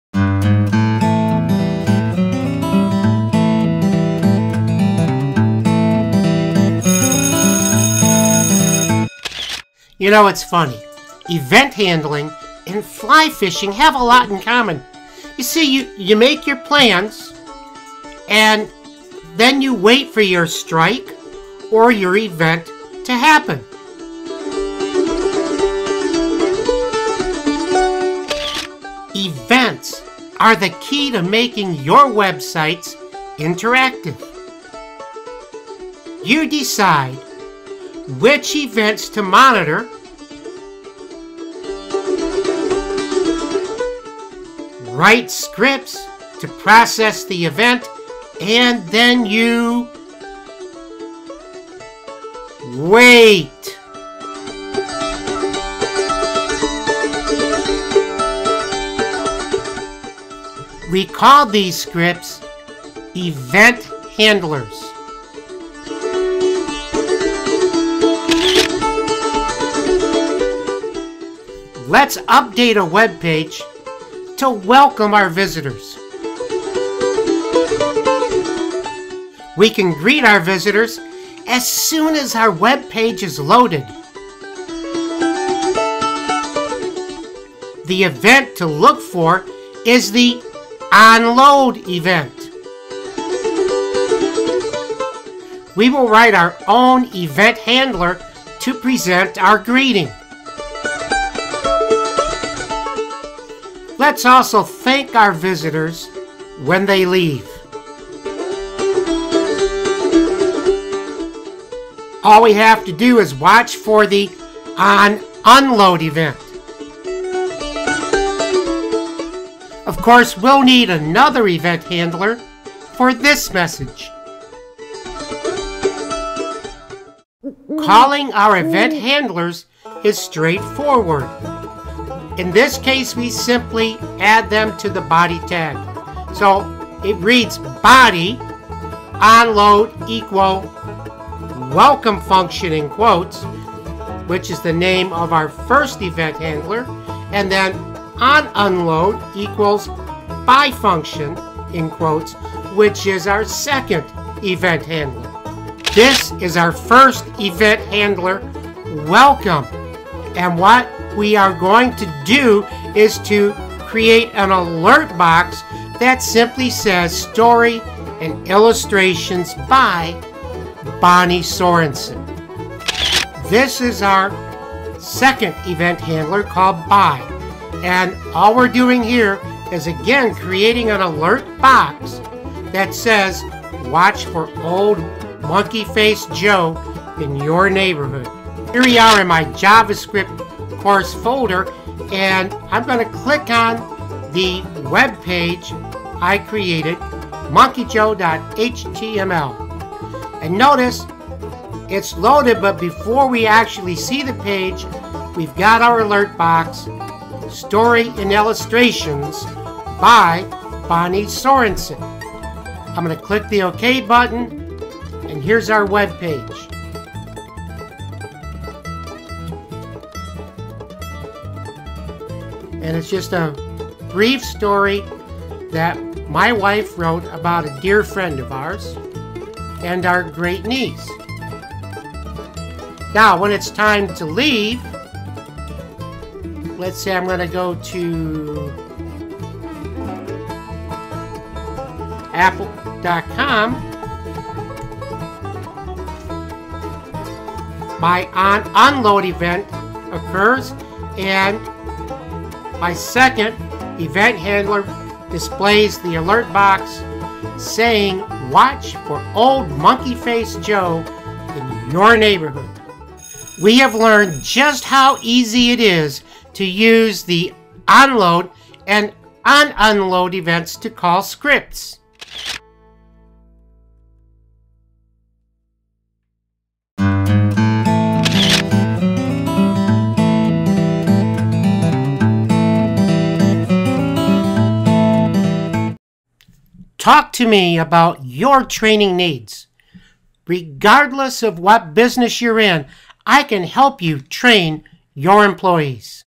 JavaScript onLoad & onUnload
Dr. Mark creates event handlers for onLoad and onUnload events.